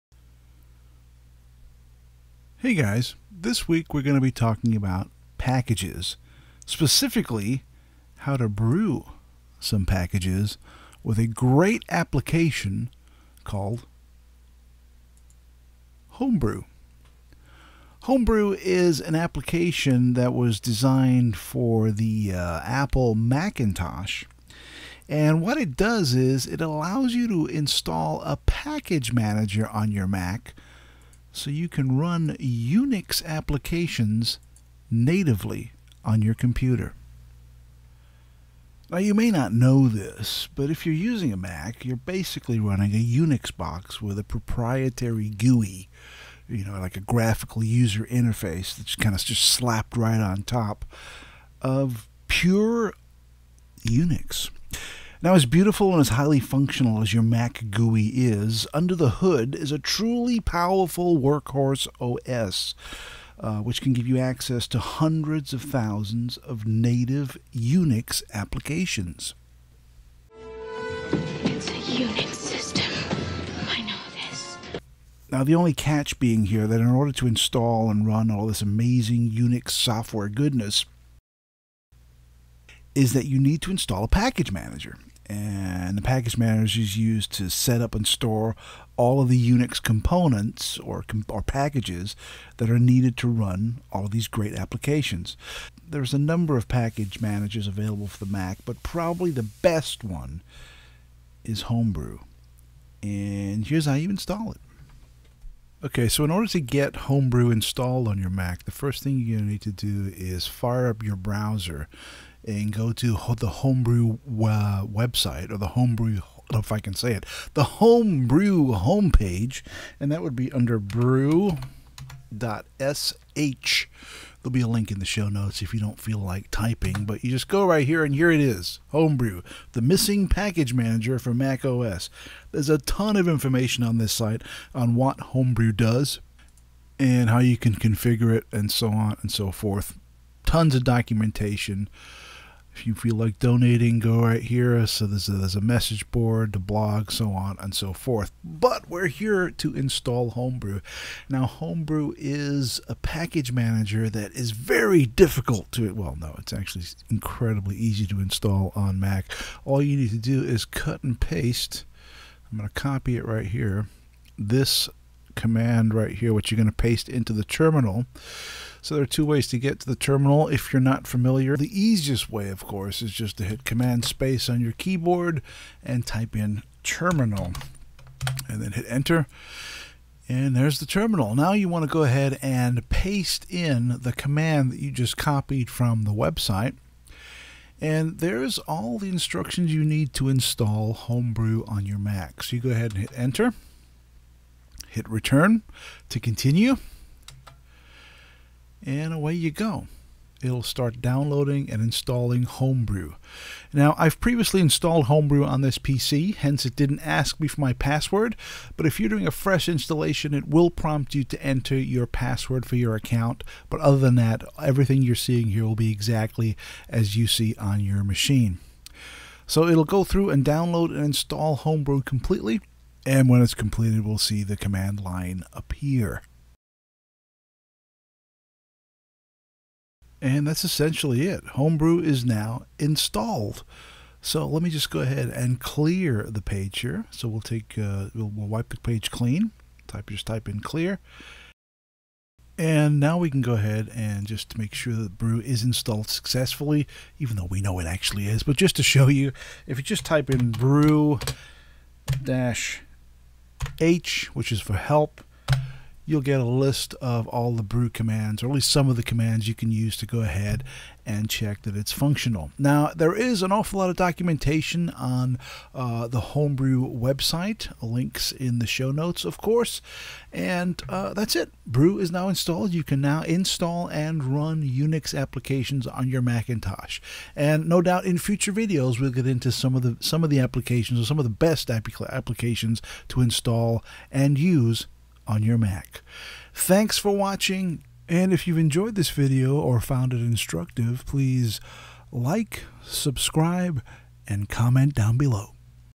Unix Apps on a Mac - HomeBrew
There are a number package managers available for the Mac, but probably the best one out there is called ‘Homebrew’ and it can be installed from the OS X TERMINAL very simply.